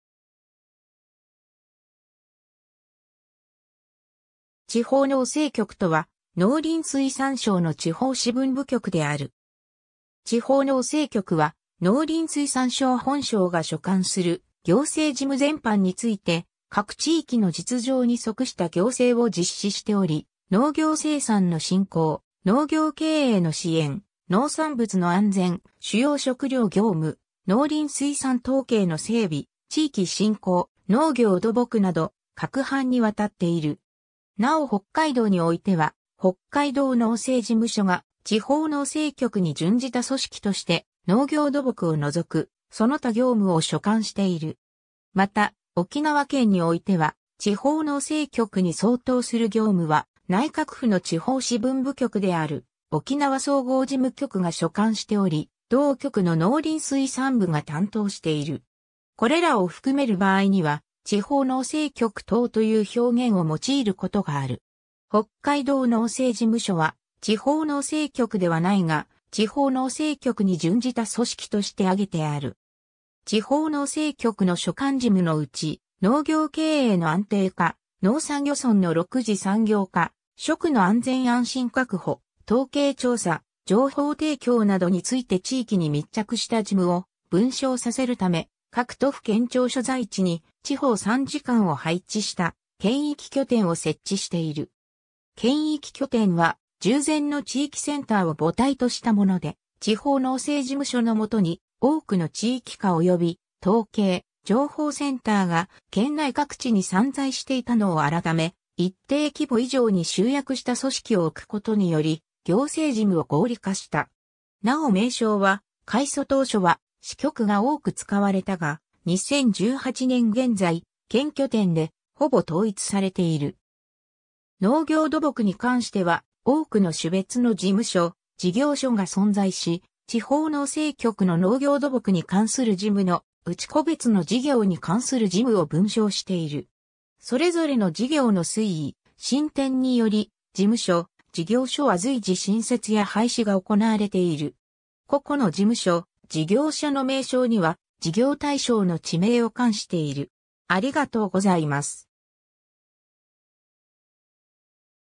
地方農政局
地方農政局（ちほうのうせいきょく）とは、農林水産省の地方支分部局である（農林水産省設置法第18条、農林水産省組織令第119条）。
 地方農政局は、農林水産省本省が所管する行政事務全般について各地域の実情に即した行政を実施しており、農業生産の振興、農業経営の支援、農産物の安全、主要食糧業務、農林水産統計の整備、地域振興、農業土木（新規農地開拓、農業水利、土壌改良等）など、各般にわたっている。
 なお北海道においては北海道農政事務所が地方農政局に準じた組織として、農業土木を除くその他業務を所管している（農業土木業務は国土交通省北海道開発局が担当）。
また沖縄県においては、地方農政局に相当する業務は内閣府の地方支分部局である沖縄総合事務局が所管しており、同局の農林水産部が担当している。
これらを含める場合には「地方農政局等」という表現を用いることがある。
 (*) 北海道農政事務所は地方農政局ではないが、地方農政局に準じた組織として挙げてある。
 地方農政局の所管事務のうち、農業経営の安定化、農山漁村の六次産業化、食の安全安心確保、統計調査、情報提供などについて地域に密着した事務を分掌させるため、各都府県庁所在地に地方参事官を配置した県域拠点を設置している。
県域拠点は従前の地域センターを母体としたもので、地方農政事務所のもとに多くの地域課および統計・情報センターが県内各地に散在していたのを改め、一定規模以上に集約した組織を置くことにより、行政事務を合理化した。
 なお名称は、改組当初は「○○支局（例:滋賀支局）」が多く使われたが、2018年現在「○○県拠点（例:滋賀県拠点）」でほぼ統一されている。
 農業土木に関しては多くの種別の事務所・事業所が存在し、地方農政局の農業土木に関する事務のうち個別の事業に関する事務を分掌している。
それぞれの事業の推移・進展により、事務所・事業所は随時新設や廃止が行われている。
個々の事務所・事業所の名称には、事業対象の地名を冠している。